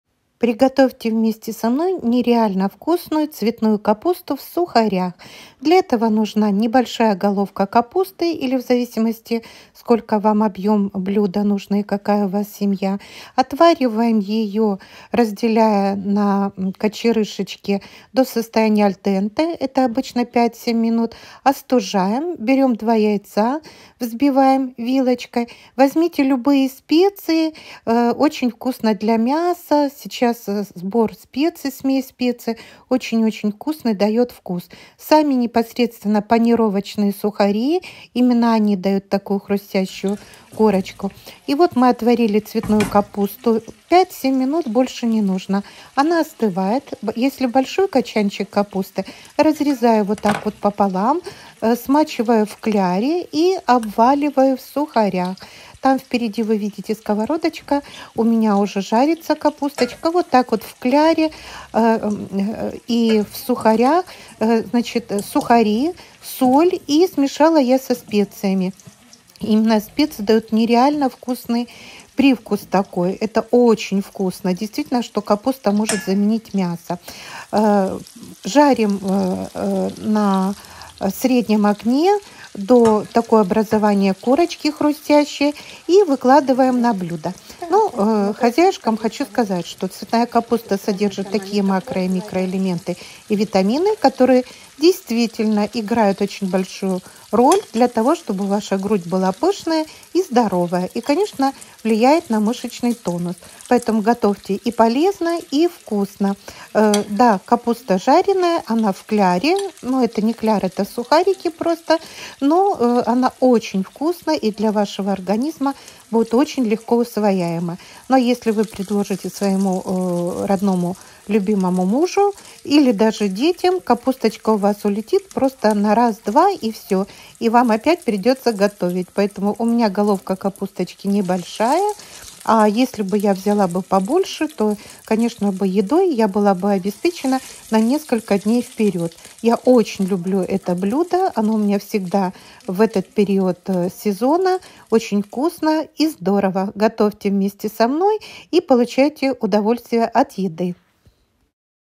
«ПЫШНАЯ ГРУДЬ!»Цветная капуста в сухарях
Цветная капуста в сухарях и специях . Полезно ! Рецепт